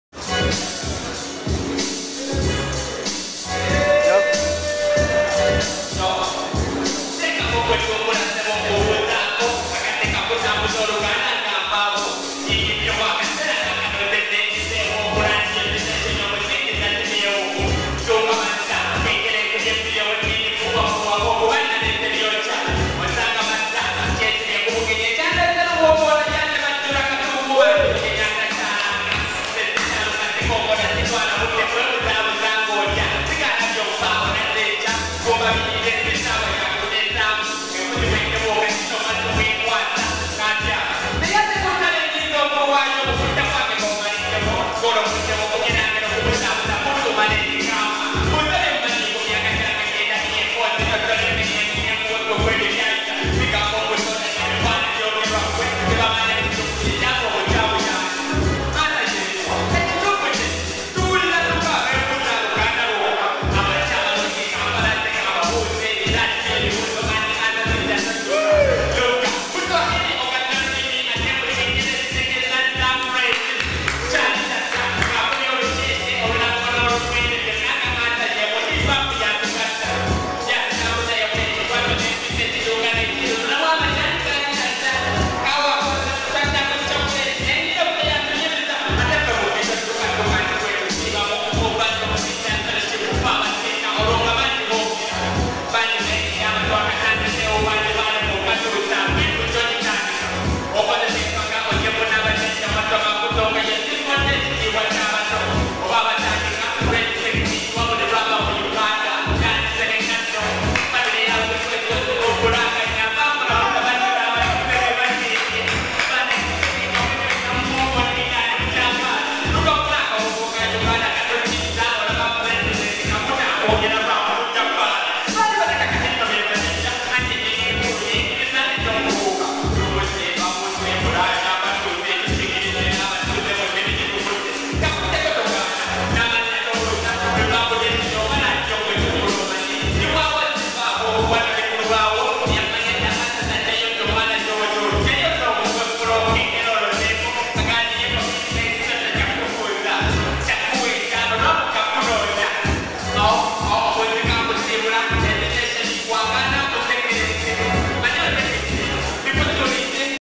Spit it Loud...St. Nelly-sade
This is just a simple perfomance by St. Nelly-sade showing the truth at the Uganda Hip-hop Lounge. Skilled and truth gifted. Check it out Now. Kikola sense.